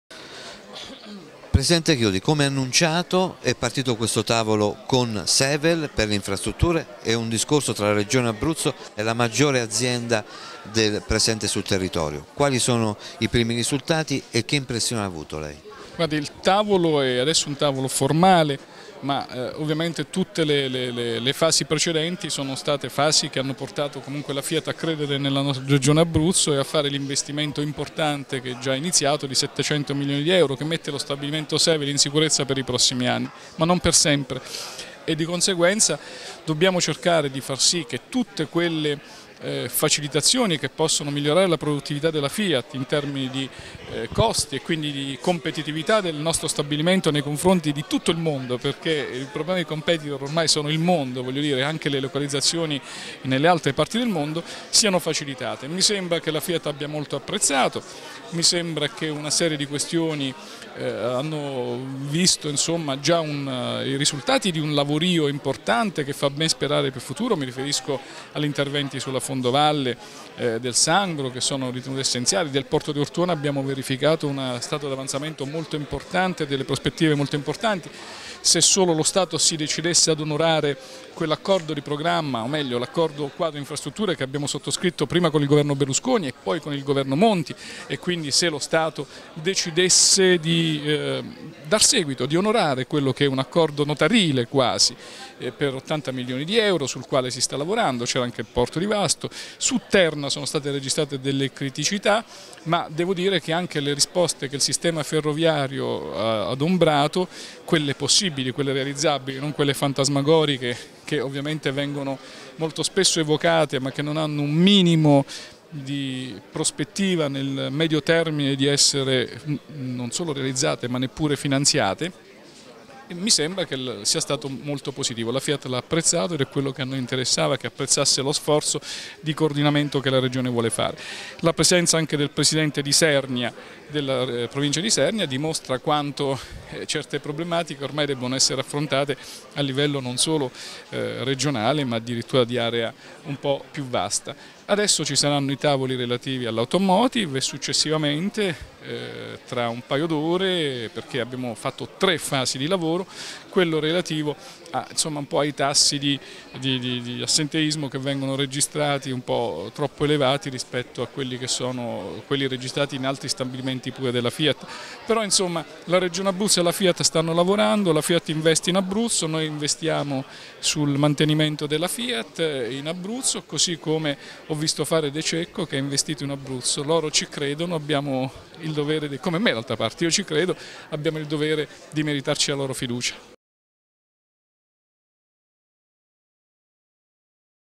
Chiodi: la Fiat investe in Abruzzo, abbiamo il dovere di meritarci la loro fiducia
Un tavolo di confronto per rafforzare l'investimento che il Gruppo Fiat ha annunciato per lo stabilimento Sevel di Atessa. E' questo l'obiettivo principale del Tavolo Infrastrutture che e' stato avviato oggi dal presidente della Regione, Gianni Chiodi, al quale partecipano il management della Fiat e della Sevel, i rappresentanti delle istituzioni comunali e provinciali interessati al progetto di sviluppo e i vertici delle aziende che forniscono servizi infrastrutturali. "La riunione di oggi - ha spiegato Gianni Chiodi - di fatto apre una collaborazione continua e importante tra la Regione Abruzzo e la Fiat che sullo stabilimento Sevel ha annunciato un investimento di 700 milioni di euro. Il Tavolo Infrastrutture dovra' agevolare questo investimento creando quelle condizioni di competitivita' cui ha fatto riferimento l'Ad Marchionne nell'incontro che abbiamo avuto ad Atessa. In sostanza - ha sottolineato Chiodi - vogliamo creare le condizioni per mettere in sicurezza l'investimento Fiat e lo stabilimento Sevel per almeno i prossimi 10 anni". Nel corso della riunione, Fiat ha illustrato alcuni problemi infrastrutturali sul territorio legati alla Ferrovia, alla viabilita' stradale e ai porti. Su questi aspetti il Tavolo ha dato prime importanti risposte. Sulla Ferrovia, e' stato annunciato l'avvio di un progetto di ammodernamento dello scalo di Fossacesia che presenta particolari criticita' soprattutto per treni merci lunghi 500 metri. Piu' difficile la situazione di collegamento sulla Tirreno-Adriatico, come ha chiesto Fiat. Sul fronte dei collegamenti stradali, Anas ha illustrato la situazione di completamento della Fondovalle del Sangro, che Fiat ritiene strategica ai fini dello smercio. Il progetto originario presenta un preventivo di spesa di 160 milioni di euro, cifre impensabili in relazione alla situazione attuale della finanza pubblica. In questo senso, il rappresentante dell'Anas ha annunciato che entro ottobre verra' stilato un nuovo progetto di fattibilita' della Fondovalle teso a ridurre i costi del precedente. Questo progetto puo' contare su un budget di partenza di 40 milioni di euro e comunque complessivamente dovrebbe ammontare a 100 milioni di euro. "La criticita' di questo progetto - ha detto Chiodi - sta nei finanziamenti. Dobbiamo fare di tutto per reperire risorse per 60 milioni di euro in grado di garantire la fattibilita' del nuovo progetto".

Il Tavolo Infrastrutturale ha inoltre affrontato il nodo dei porti, che Fiat ritiene essenziale per il trasporto delle merci pesanti. Le attenzioni si sono concentrate sui porti di Ortona e Vasto, porti con caratteristiche diverse che possono soddisfare, con alcuni importanti arricchimenti, le esigenze di Sevel. Su questo aspetto il presidente Chiodi si e' detto fiducioso sul progetto di potenziamento del porto di Ortona. "Stiamo aspettando - ha detto - che lo Stato eroghi quanto promesso per il porto di Ortona: quei 90 milioni di euro che sono stati sottoscritti in due diversi Protocolli d'intesa che la Regione Abruzzo ha sottoscritto con due governi nazionali diversi negli ultimi quattro anni. Il progetto di potenziamento del porto esiste, stiamo solo aspettando le risorse per finanziarlo". Ancora sul porto di Ortona, il presidente della Provincia di Chieti, Enrico Di Giuseppantonio, ha annunciato che prima di Natale sara' completata e dunque attiva la bretelle stradale che collega il porto con il casello autostradale dell'A14. Contributi importanti alle richieste della Fiat sono arrivate anche da Terna per i problemi legati all'erogazione di energia elettrica , dalla Saga per l'aeroporto d'Abruzzo e da Confindustria Abruzzo. Il Tavolo Infrastrutture tornera' a riunirsi tra due mesi. "Un periodo sufficiente per accertare - ha concluso il presidente Gianni Chiodi - se quanto affermato in questo tavolo ha iniziato a prendere corpo". Subito dopo il tavolo delle Infrastrutture, sempre per volere del presidente della Regione, sono stati attivati altri due tavoli di confronto con Sevel: quello dell'Automotive, con il progetto di realizzazione del campus con i fondi Fas, e quello della sicurezza dei lavoratori sui luoghi di lavoro.